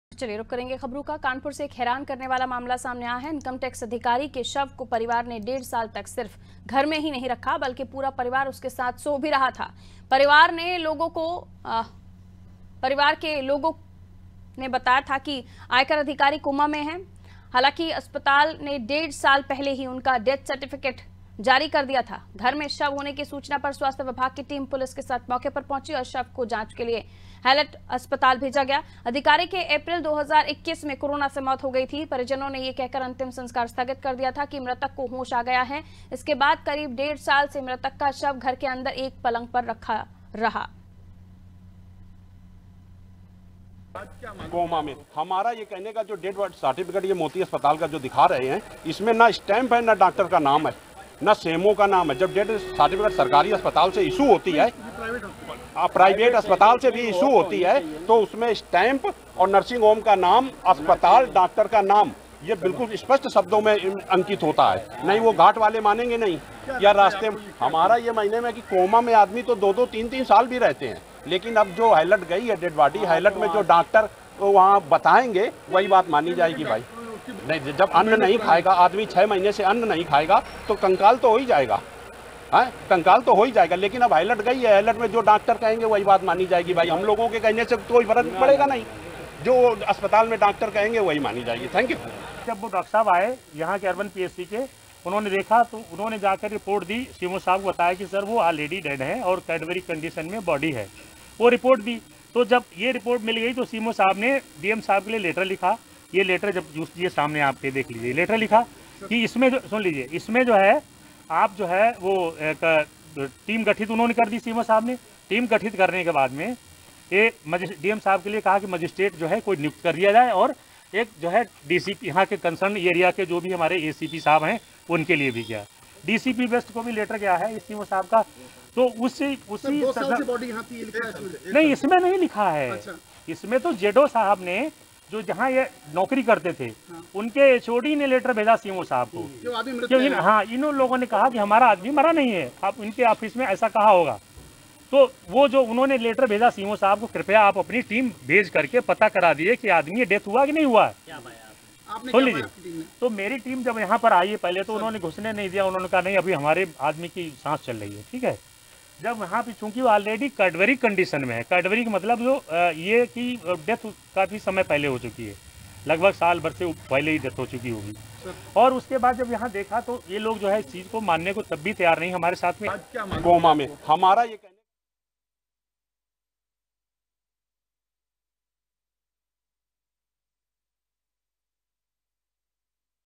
Kanpur : शव के साथ परिवार के डेढ़ साल | UP HINDI NEWS
कानपुर से एक हैरान करने वाला मामला सामने आया है... इनकम टैक्स अधिकारी के शव को परिवार ने डेढ़ साल तक सिर्फ घर में ही नहीं  रखा...  बल्कि पूरा परिवार उसके साथ सो भी रहा था... परिवार ने लोगों को बताया था कि आयकर अधिकारी कोमा में है... हालांकि, अस्पताल ने डेढ़ साल पहले ही उनका डेथ सर्टिफिकेट जारी कर दिया था... घर में शव होने की सूचना पर स्वास्थ्य विभाग की टीम पुलिस के साथ मौके पर पहुंची... और शव को जांच के लिए हैलट हॉस्पिटल भेज दिया... अधिकारी की अप्रैल 2021 में कोरोना से मौत हो गई थी... परिजनों ने यह कहकर अंतिम संस्कार स्थगित कर दिया था... कि मृतक को होश आ गया है... इसके बाद करीब डेढ़ साल से मृतक का शव घर के अंदर एक पलंग पर रखा रहा था...